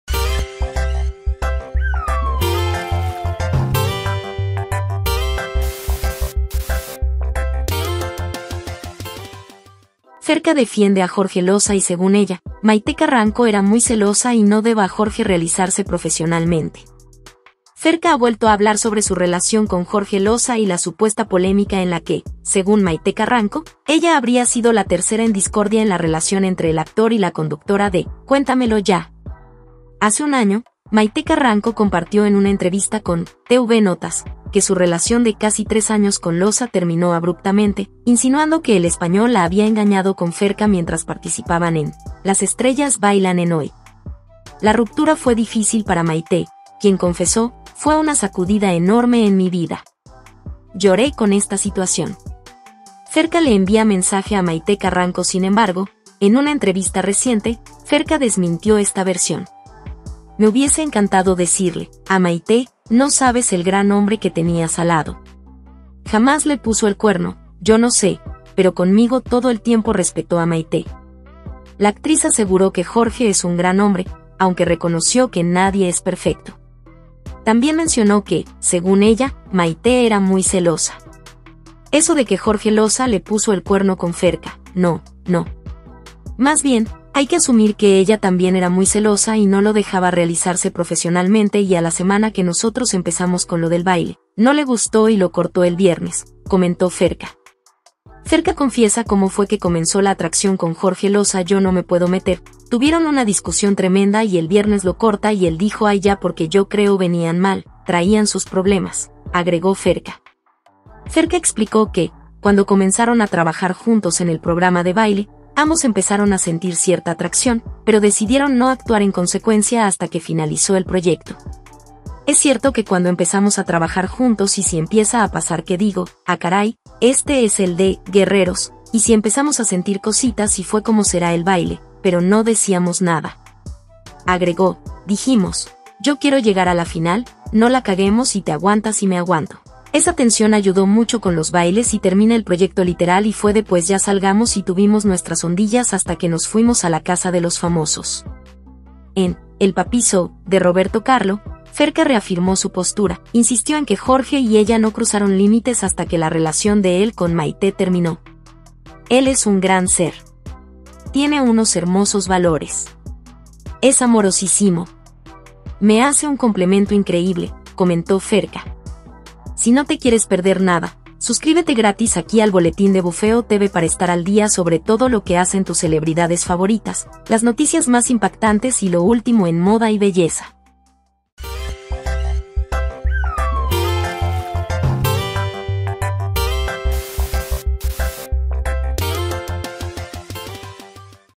Ferka defiende a Jorge Losa y envía mensaje a Mayte Carranco No sabes el gran hombre que tenías a tu
Ferka defiende a Jorge Losa y según ella, Mayte Carranco era muy celosa y no deba a Jorge realizarse profesionalmente.
Ferka ha vuelto a hablar sobre su relación con Jorge Losa y la supuesta polémica en la que, según Mayte Carranco, ella habría sido la tercera en discordia en la relación entre el actor y la conductora de ‘Cuéntamelo Ya!’
Hace un año, Mayte Carranco compartió en una entrevista con ‘TVNotas’ que su relación de casi tres años con Losa terminó abruptamente, insinuando que el español la había engañado con Ferka mientras participaban en ‘Las estrellas bailan en Hoy’. La ruptura fue difícil para Mayte, quien confesó: “Fue una sacudida enorme en mi vida. Lloré con esta situación”.